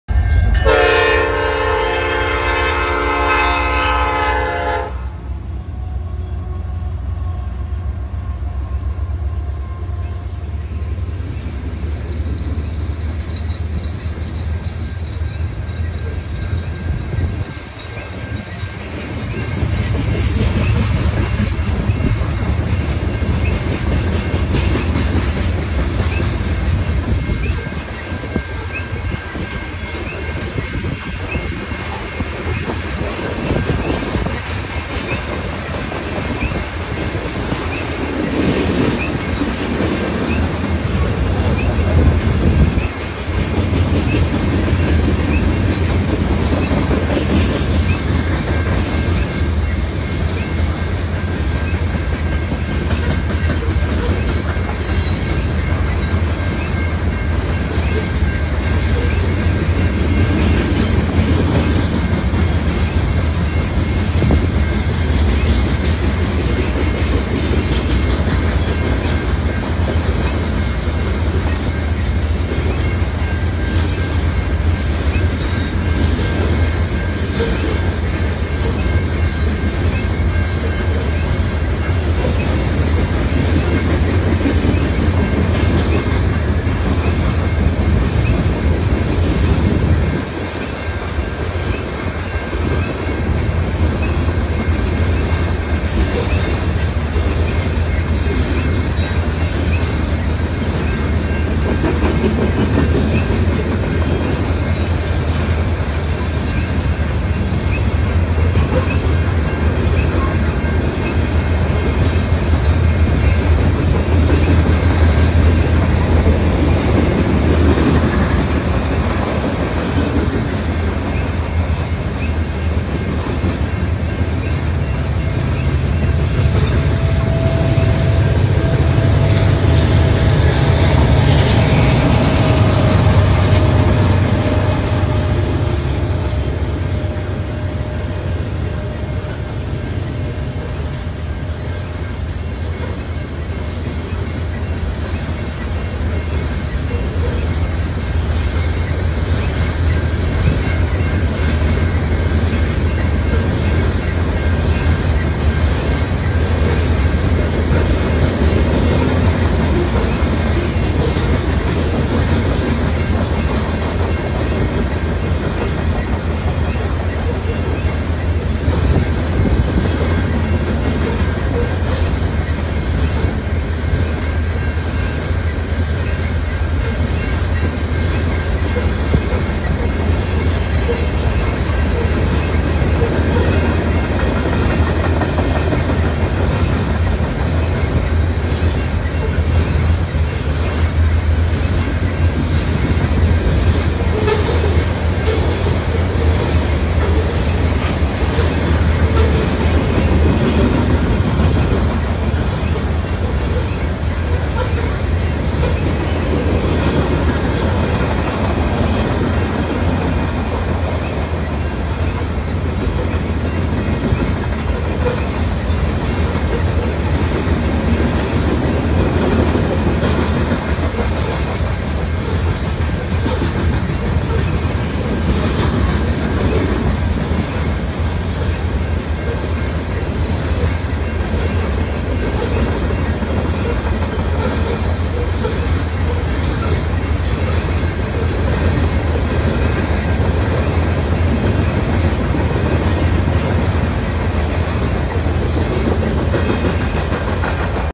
CN 8873 in Hammond heading into New Orleans
Here’s a video on of CN 8873 with BNSF 6875 going southbound towards CN’s yard in New Orleans. The Mid DPU were Northfolk Southern engines 1115 and 9564. Enjoy